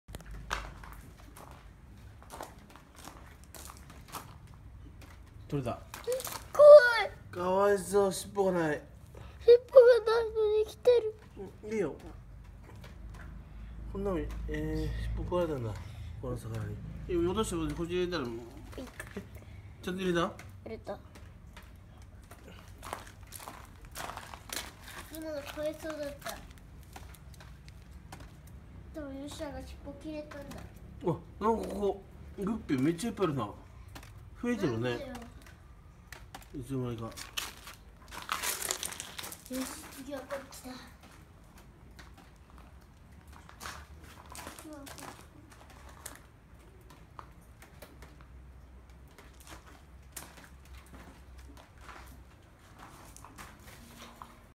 MARUMIYAよしや /焼き芋&グッピー捕り♪
外は小雨が降っているので、ガレージで焼き芋しながらグッピー捕り。
ヨシヤが管理？しているガレージのチビッコ池♥
エビ、メダカ、グピー、ヤゴ、ハゼ、カニ等　色々います。